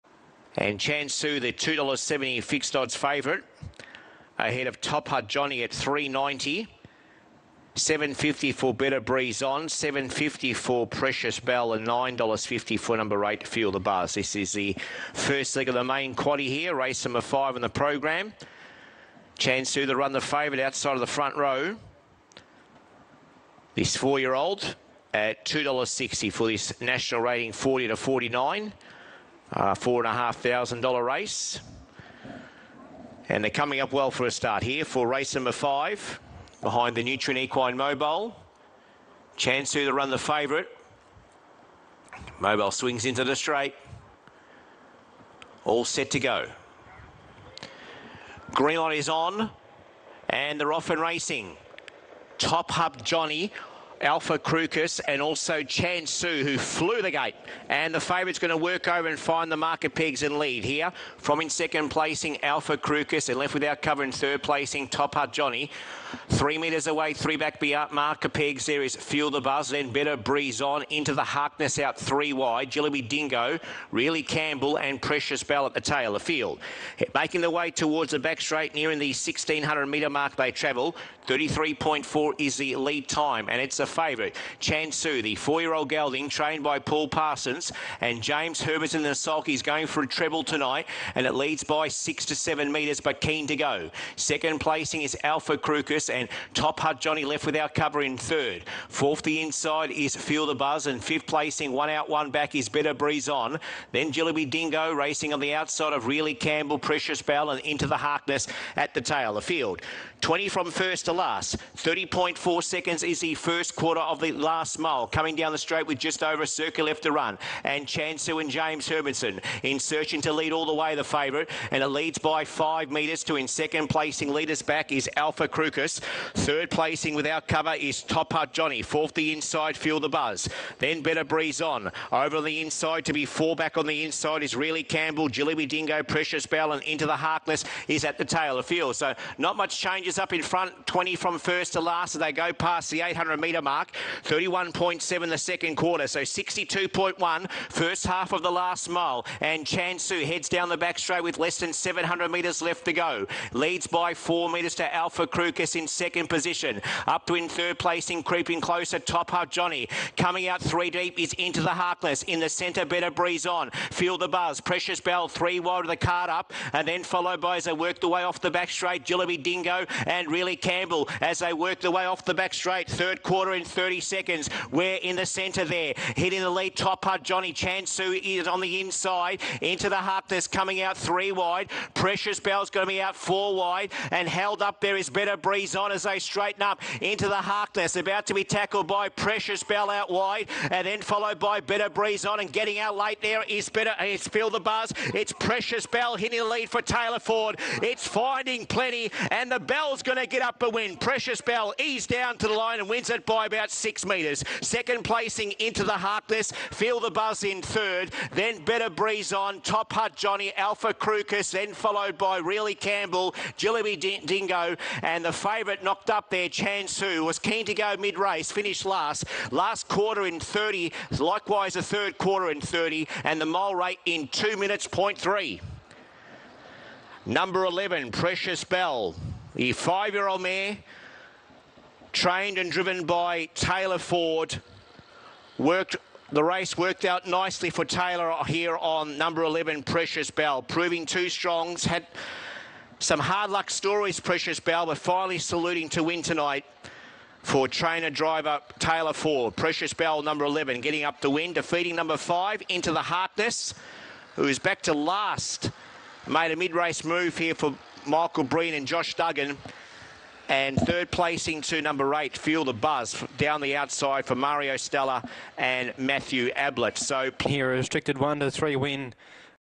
Harness Racing- Cranbourne- Race 5- May 26, 2024
5  7:27pm LATHER UP STANDING @ WOODLANDS STUD PACE 2080M
$4,500 NR 40 to 49. PBD/NR. PACERS. Mobile 1ST LEG QUADDIE, 1ST LEG DAILY DOUBLE

Horse
1 JAY BEE (Em 1)       SCR
2 ALPHA CRUCIS                 FR1
3 BETTOR BREEZE ON         FR2
4 TOPHUT JOHNY            FR3
5 INTO THE HARKNESS   FR4
6 DEFYING THE ODDS          SCR
7 CHANSU                         FR5
Second Row
8 FEEL THE BUZZ              SR1
9 JILLIBY DINGO                    SR2
10 REALLY CAMPBELL         SR3
11 PRECIOUS BELLE NZ   SR4

Results
Pl  Horse  Prize-money  Row & Br TAB #   Trainer   Driver (C = Concession) Mgn (m) Starting odds  Stewards'  Comments
1 PRECIOUS BELLE NZ  $ 2,250   Sr4 11 Taylor Ford Taylor Ford  $ 8.00   PRS OPS 8 D/F
BAY MARE 5 by BETTORS DELIGHT USA out of OUR PRECIOUS ROSE NZ (FALCON SEELSTER USA)
Owner(s): Boots Properties Racing Pty Ltd
Breeder(s): Old Ridge Services No 4 Ltd
2 INTO THE HARKNESS  $ 700   Fr4 5 Michael Breen Josh Duggan 7.60 $ 11.00   CWE RTR 10 3WL
3 FEEL THE BUZZ  $ 460   Sr1 8 Mario Stella Matthew Ablett 9.60 $ 9.50   VXAH VXLS 5
4 TOPHUT JOHNY  $ 250   Fr3 4 Ross Graham Chris Alford 12.10 $ 4.20   GS 2
5 BETTOR BREEZE ON  $ 140   Fr2 3 Michael Cole Jodi Quinlan 12.60 $ 7.50   4
6 ALPHA CRUCIS  $ 140   Fr1 2 Ken Tippet Joshua Smyth 23.60 $ 10.00   GS 3 INC
7 REALLY CAMPBELL  $ 140   Sr3 10 Justin Campbell Justin Campbell 31.50 $ 61.00   OPS D/CSU 7
8 JILLIBY DINGO  $ 140   Sr2 9 Terry Scanlon Michael Bellman 53.30 $ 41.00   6 TIRE LCRC
9 CHANSU  $ 140   Fr5 7 Paul Parsons James Herbertson 55.10 $ 2.50 fav  GS L 1 PH TIRE WI QUER VXAR NAR
Scratchings
JAY BEE 1
DEFYING THE ODDS 6
Track Rating: GOOD Gross Time: 2:35:5 Mile Rate: 2:00:3 Lead Time: 33.4
First Quarter: 30.4 Second Quarter: 31.7 Third Quarter: 30 Fourth Quarter: 30
Margins: 7.6m x 1.9m